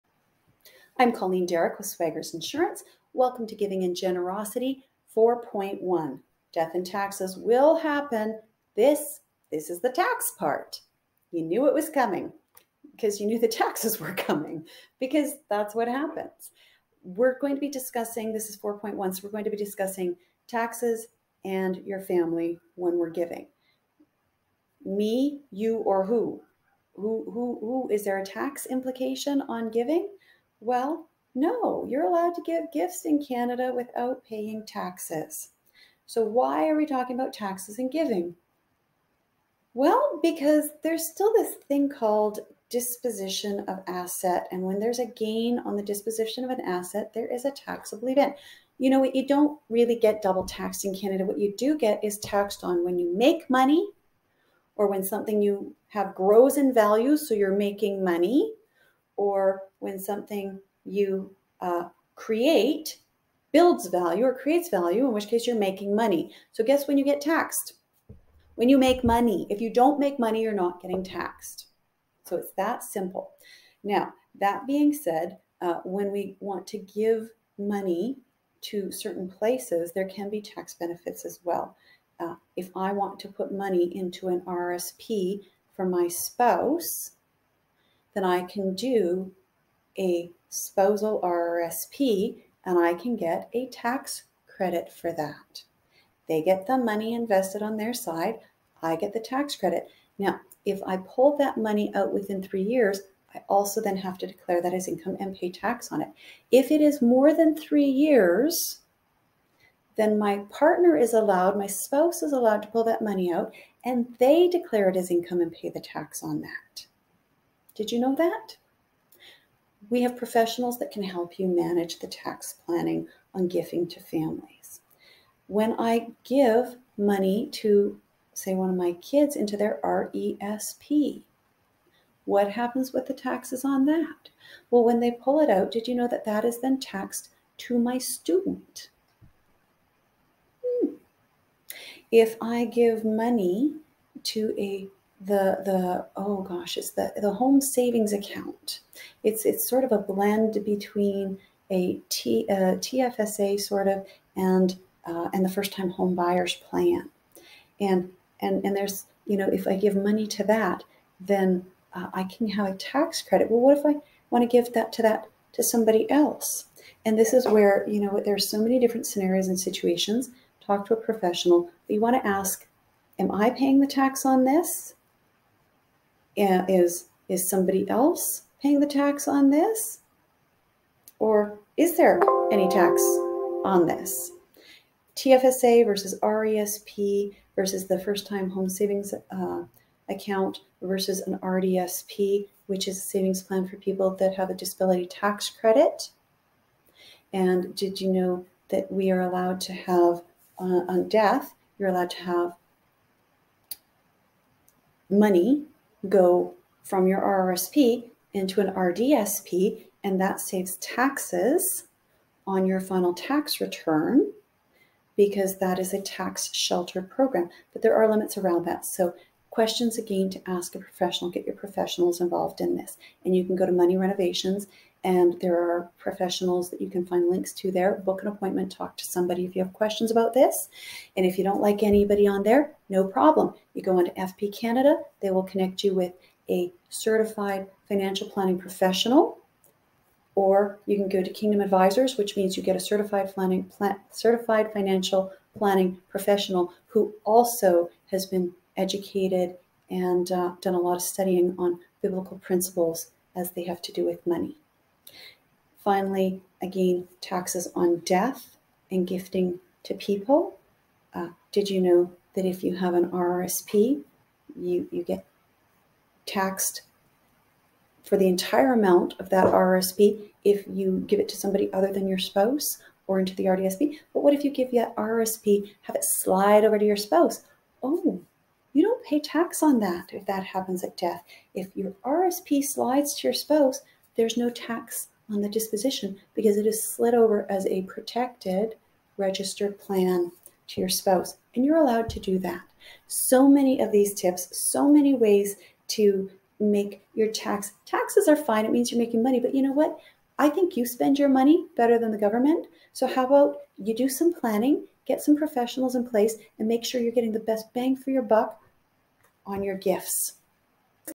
Giving & Generosity 2023 4 1
Module 4, Episode 1
Taxes, taxes and more taxes.  Who pays and when?  A starting point for considerations and which professionals you should check with before you give to family.